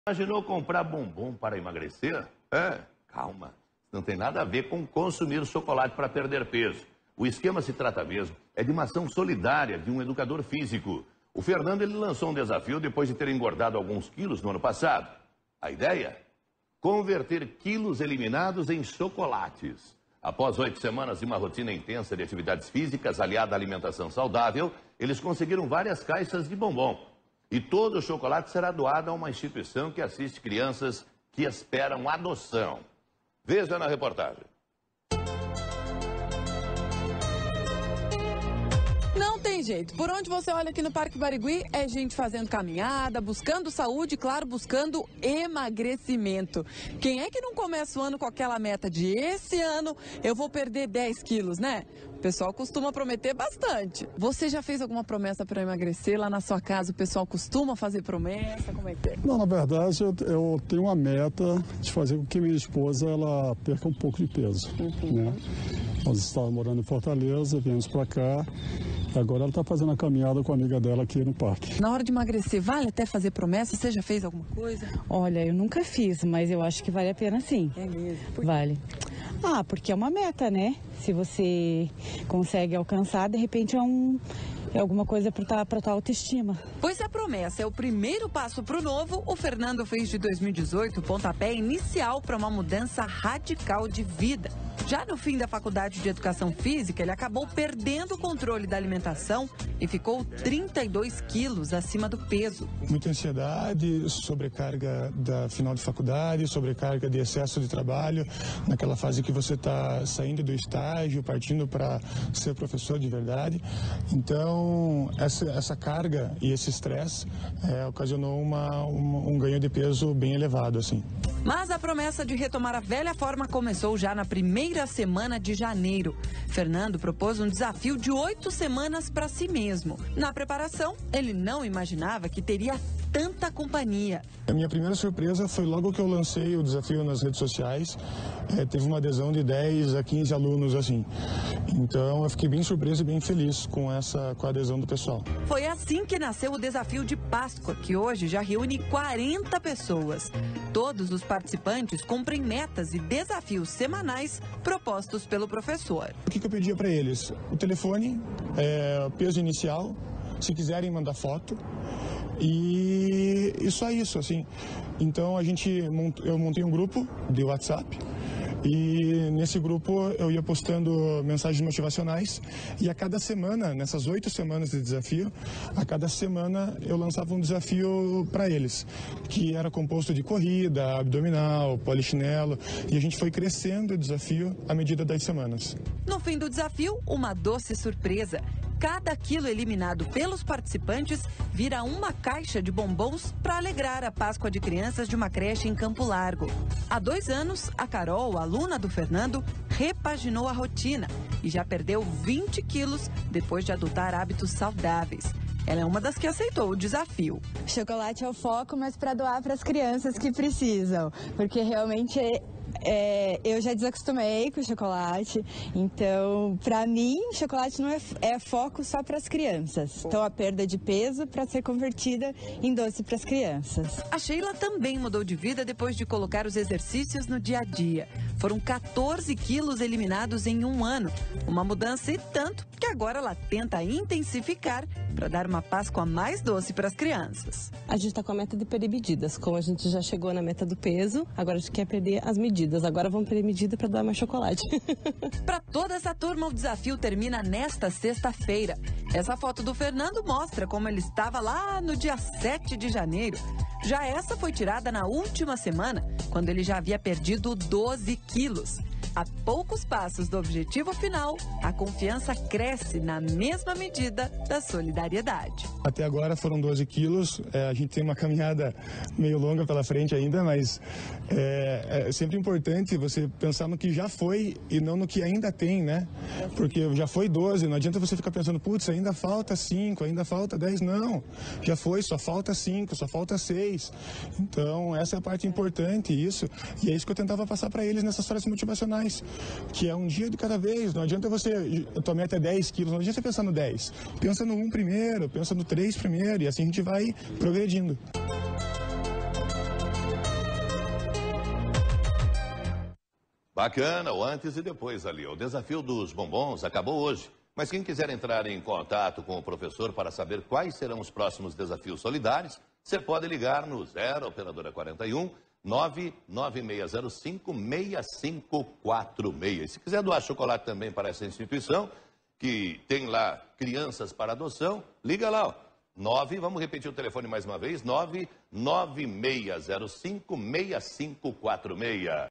Desafio para emagrecer ajuda crianças carentes
Um educador físico lançou um desafio depois de ter engordado alguns quilos, a cada quilo perdido uma caixa de chocolate será doado a uma instituição de adoção infantil. Depois de oito semanas de uma rotina intensa com atividades físicas aliadas a alimentação saudável, ele conseguiu várias caixas de bombom.